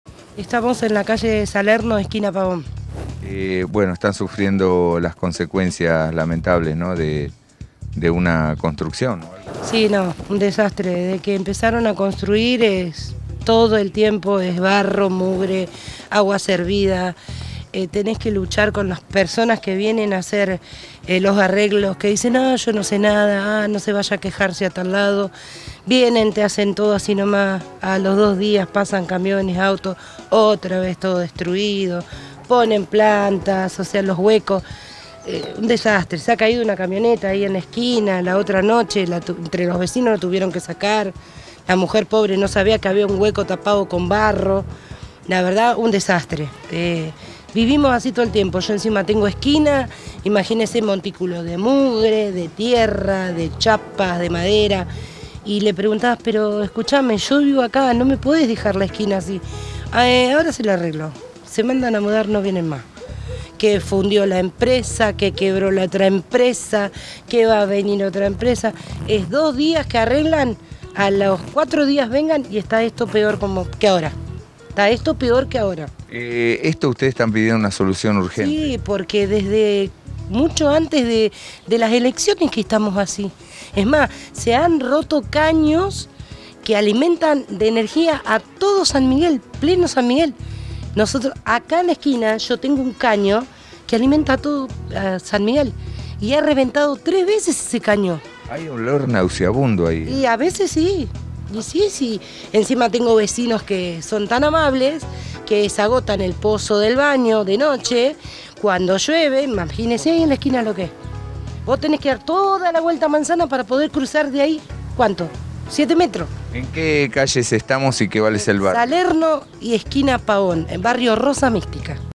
San Miguel
Temen una tragedia por dos crateres en la calle Salerno y Pavon. Obra hidraulica quedó sin terminar y se hundio el asfalto. Nota: Vecina del Barrio Maria Rosa Mistica